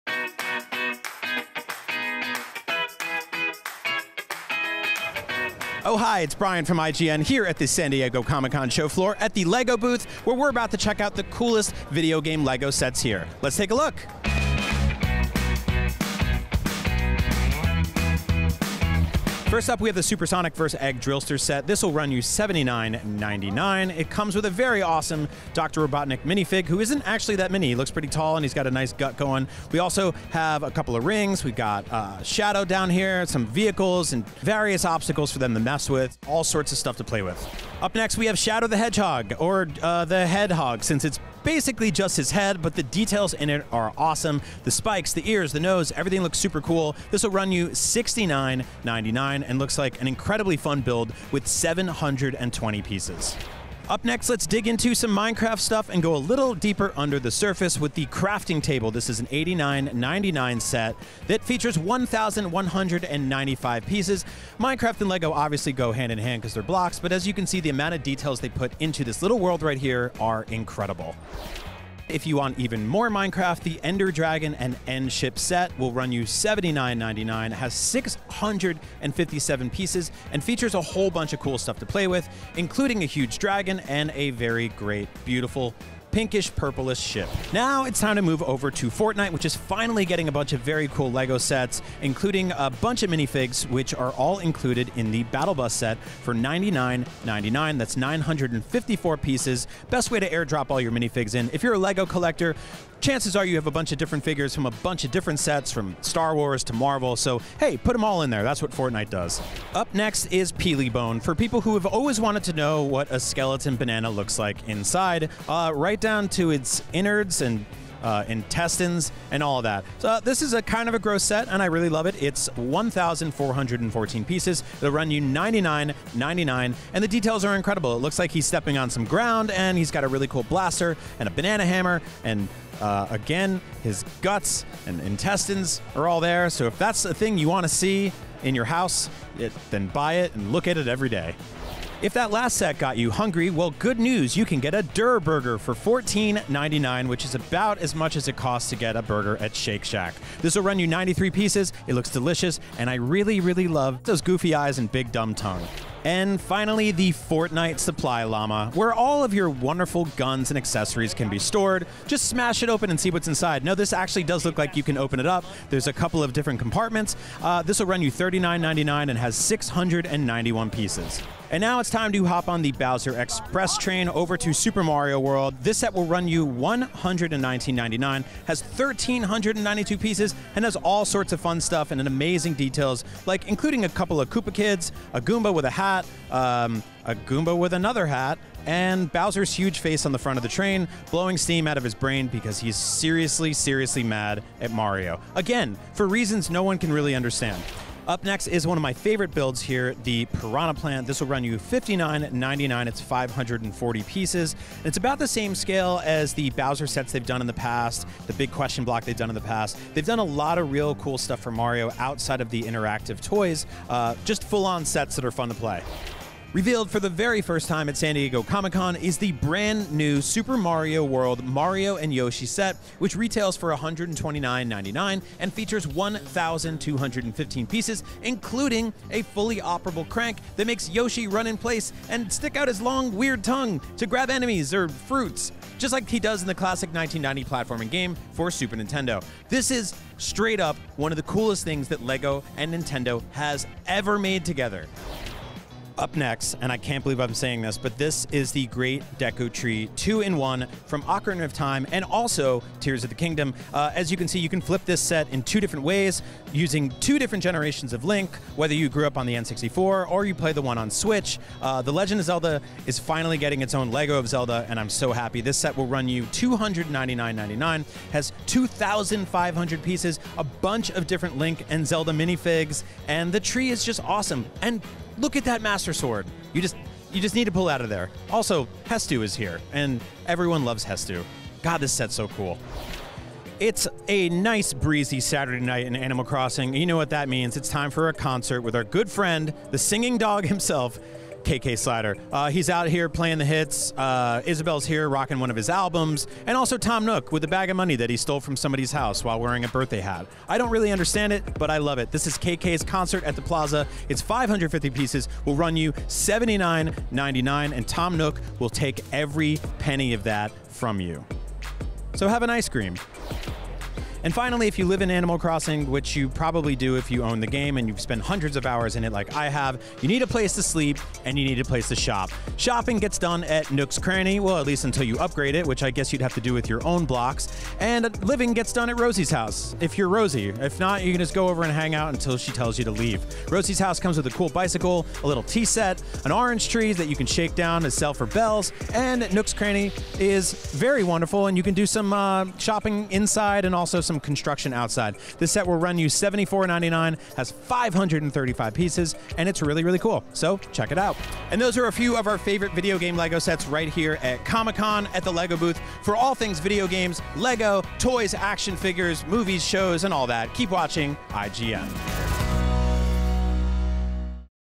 Giant Lego Yoshi With Working Tongue & More | Comic Con 2024
Ready for a large, 1200 piece Yoshi from Super Mario World built entirely out of Legos that's fitted with a working tongue? We are. The Lego Booth at San Diego Comic-Con 2024 has tons of awesome video game IPs this year and we're there to walk you through the best them. Sonic, Super Mario World, The Legends of Zelda, and Animal Crossing are among the few of elaborate sets based on these iconic video/ games.